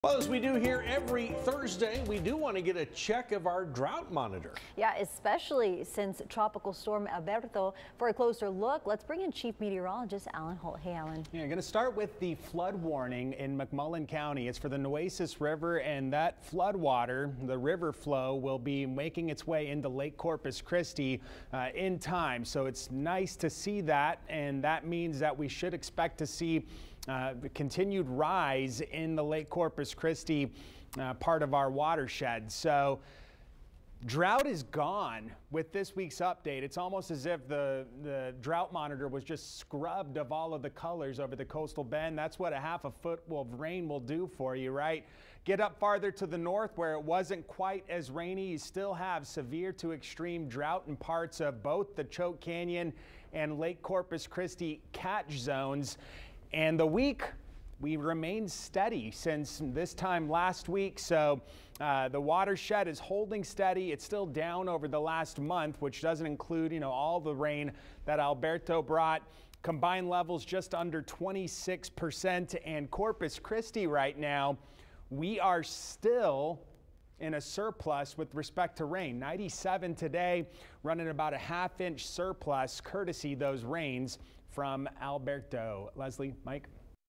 Watershed update a week after Alberto's heavy rains
Corpus Christi's combined lake level is at 25.97 percent. Falling under 20 percent capacity would initiate Stage 3 water restrictions.